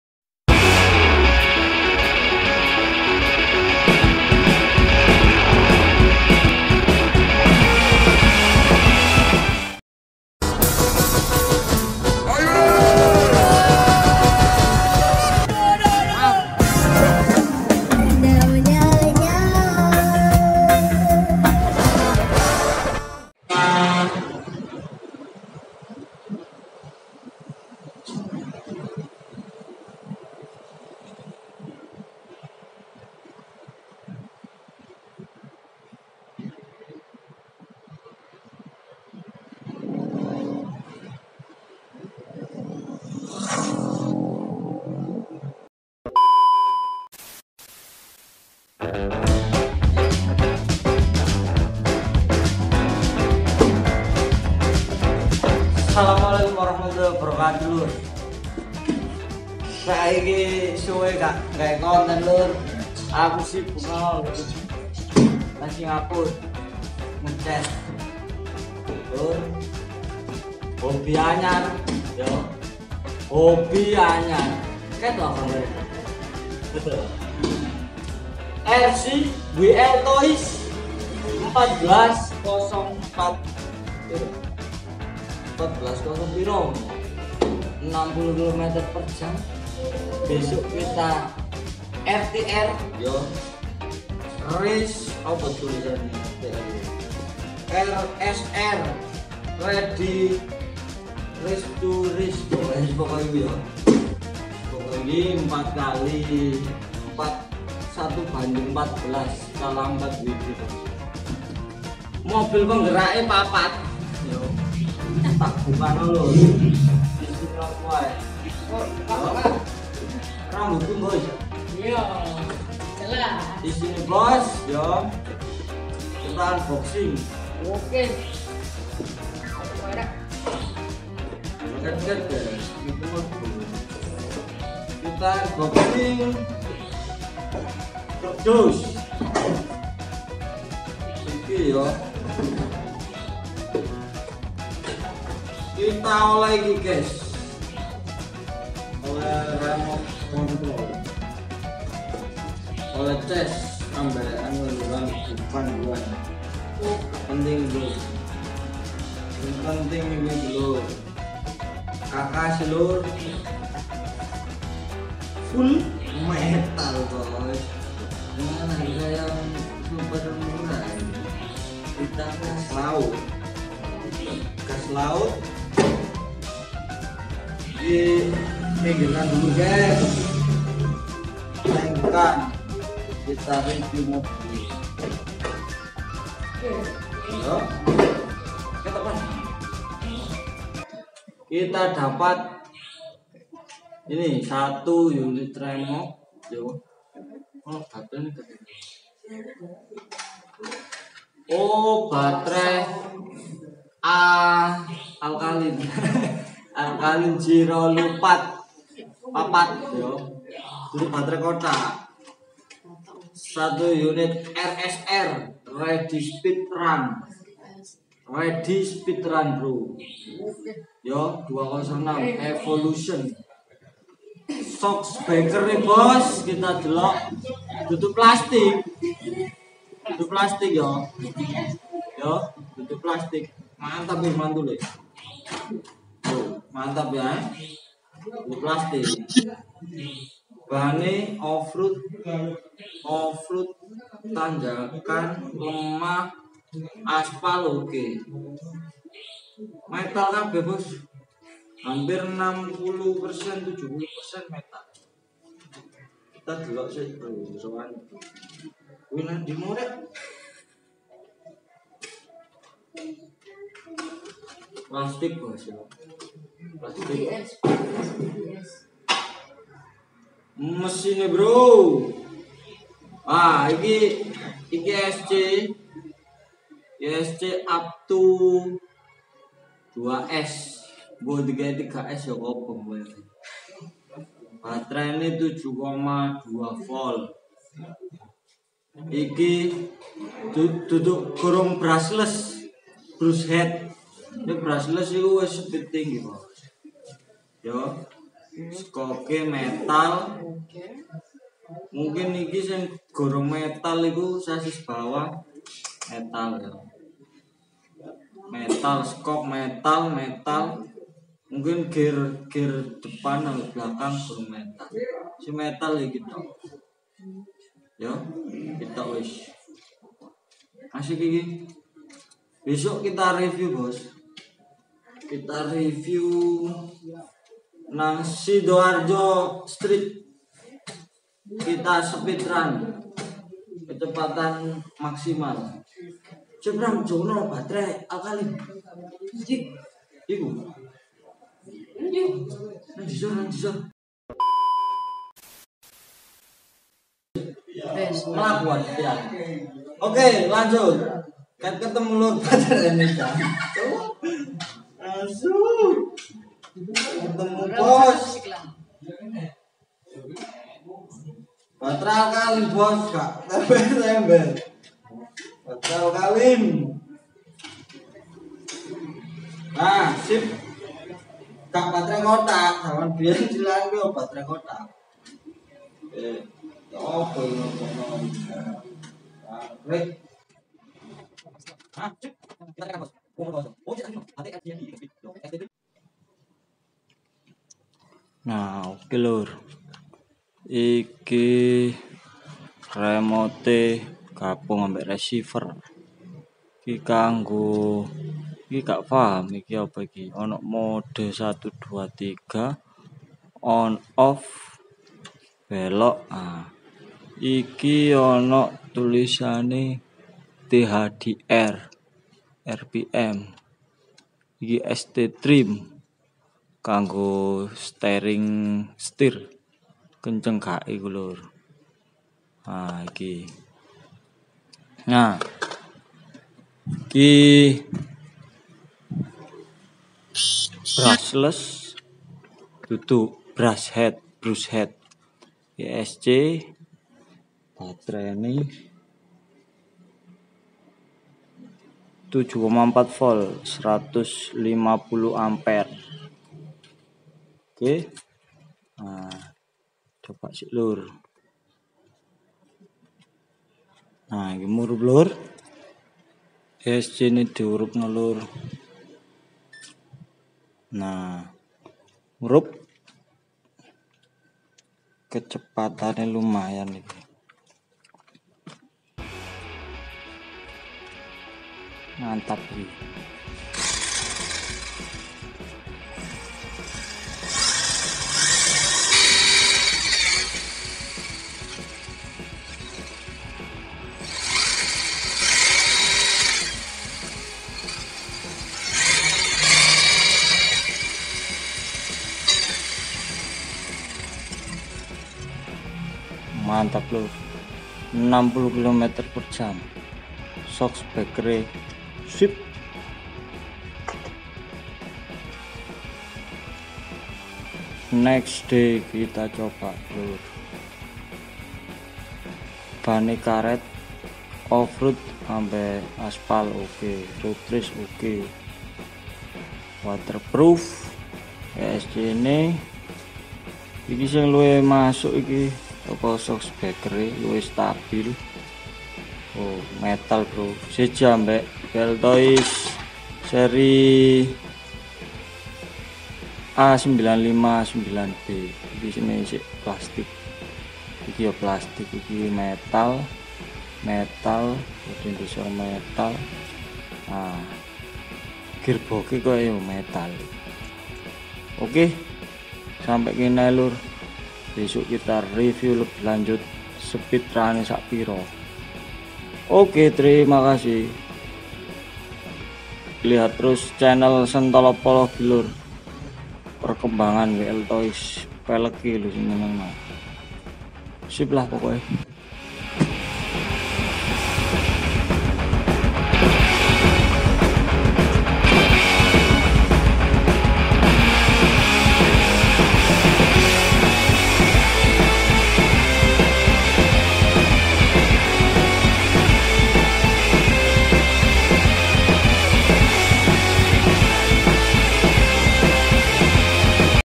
WLTOYS 144001 RSR 1:14 speed ran unboxing sepek gahar untuk hoby RC - SENTOLOP POLOGI
kali ini kita review RC hoby maaf lur bila ada penjelasan yang keliru masih pemula, rc wl toys 144001 RSR driving racing speed 60 km, unboxing 4wd rejomended setabil dapur pacuh sangar... yang paling asik suspensi dan kendali sangat setabil lur.